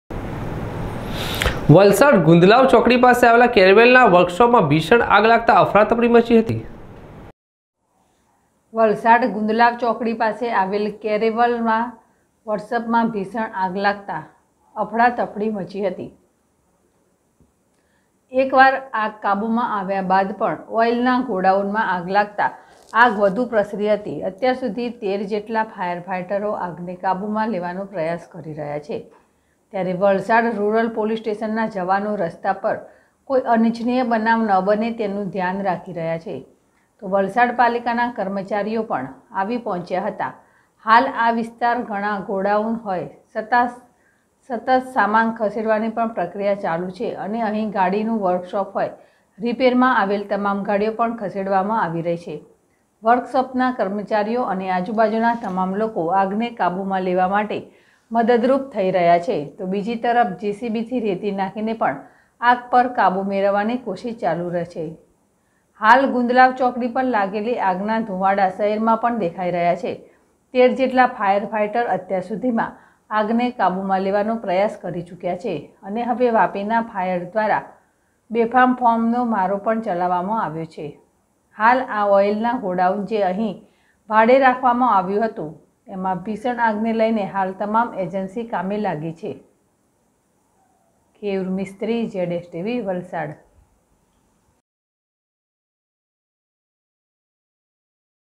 વલસાડ : ગુંદલાવ નજીક મોટરકારના વર્કશોપમાં લાગી ભીષણ આગ,
વલસાડ : ગુંદલાવ નજીક મોટરકારના વર્કશોપમાં લાગી ભીષણ આગ,પોલીસ સહિત ફાયર ફાઇટરો દોડ્યા